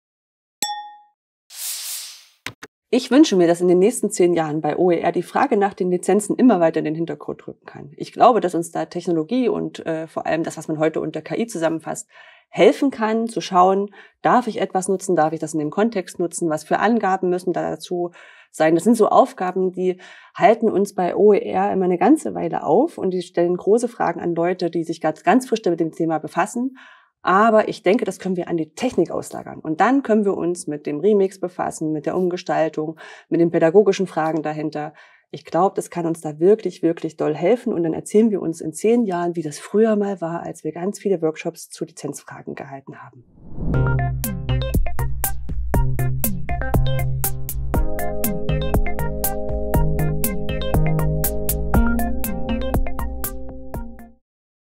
10 Jahre BFB – Anja Lorenz
Anja Lorenz These zu 10 Jahre Bündnis Freie Bildung: "Maschinelles Lernen kann helfen, die komplizierten Regeln zu Urheberrechten in offenen Bildungsressourcen zu vereinfachen. In Zukunft könnten KI-Systeme automatisch Lizenzfragen lösen und alternative Inhalte vorschlagen, was Bildung zugänglicher macht."

Vielleicht wird dies mit einem „konstruktiven Uploadfilter" umsetzbar, der wie folgt aussehen könnte: Eine Person möchte einen Kurs erstellen und diesen offen lizenzieren.  In vielen Fällen fühlt sich diese Person durch die Unsicherheit über die Rechtslage ausgebremst: Kann der Kurs und alle darin enthaltenen Materialien offen lizenziert werden oder sind sie bereits urheberrechtlich gesichert?
Eine Lösung hierfür wäre die Prüfung der Materialien via KI. Falls es keine offenen Materialien gibt, kann die KI alternative Materialien vorschlagen, z. B. alternative Bilder und Graphiken. Auf der einen Seite führt dies zu einer Arbeitserleichterung für die Kurserstellenden und reduziert auf der anderen Seite Unsicherheiten. Auf diese Weise sind mehr Leute bereit Kurse in einem offen lizenziertem Format zu veröffentlichen. Gleichzeitig führt dies zu einer Entlastung der OER-Plattformmanager.

Das Video entstand im Rahmen des OERcamps 2024. In einer Abendveranstaltung wurde das 10jährige Jubiläum des Bündnis Freie Bildung gefeiert. Aus diesem Grund schaute das OERcamp zurück, aber vor allem nach vorne und fragt: Was fordern/planen/fragen/wünschen sich BFB-Akteur:innen in Sachen Bildung für eine offene, digitale Gesellschaft für die nächsten 10 Jahre?